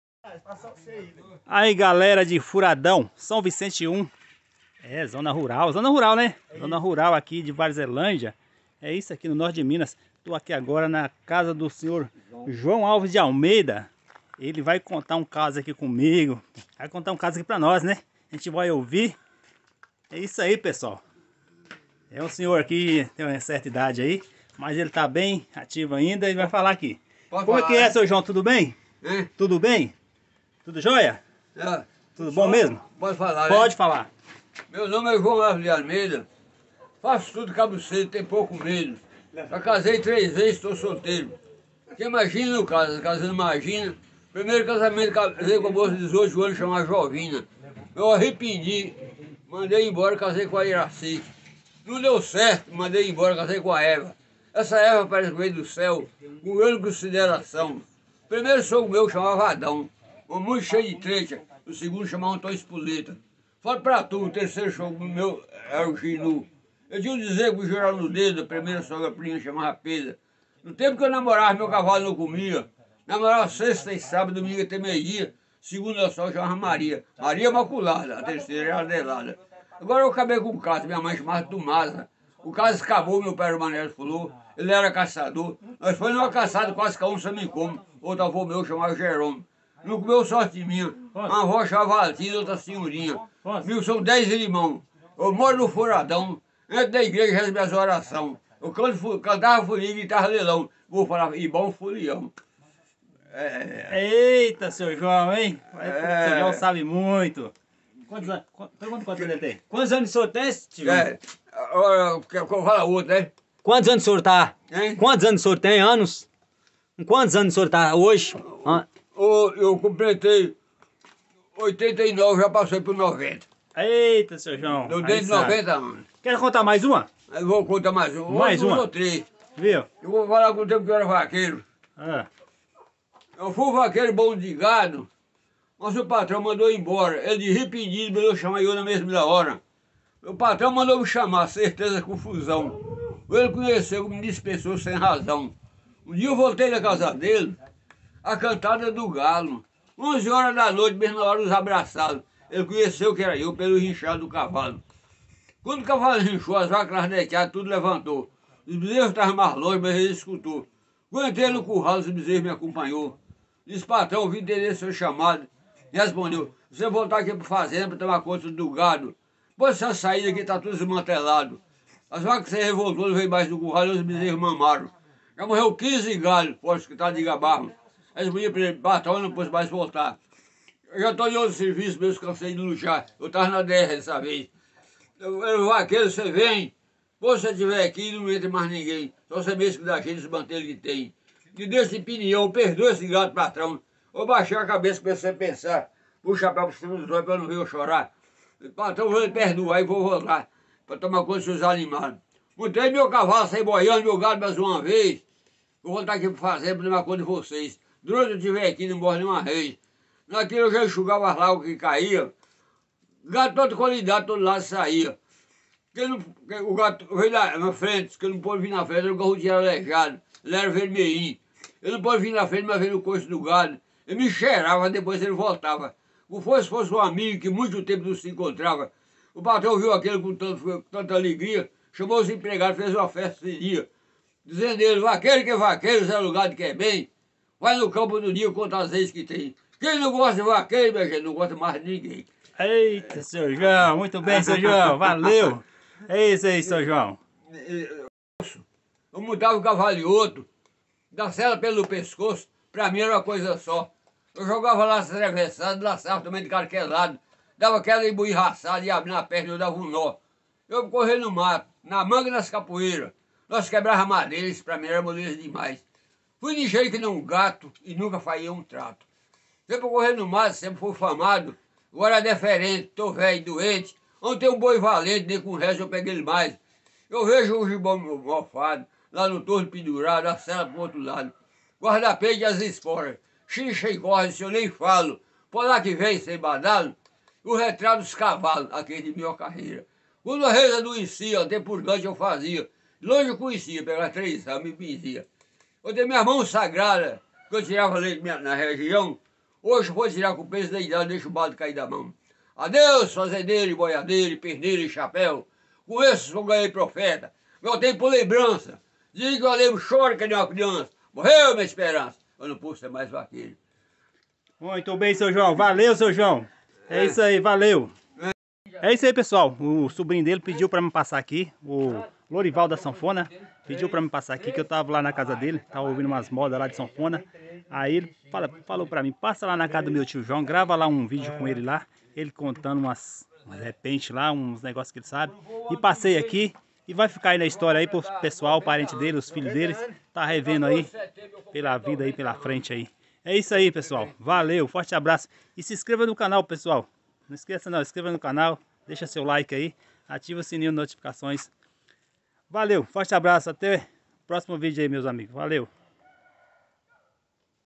Senhor joão alves com 90 anos de idade morador de furadão vai Contar uns versos
esse video foi um pedido de lourival lima

AJUDE  A MANTER O CANAL COM MAIS CONTEÚDO
Faça uma doação Via PagSeguro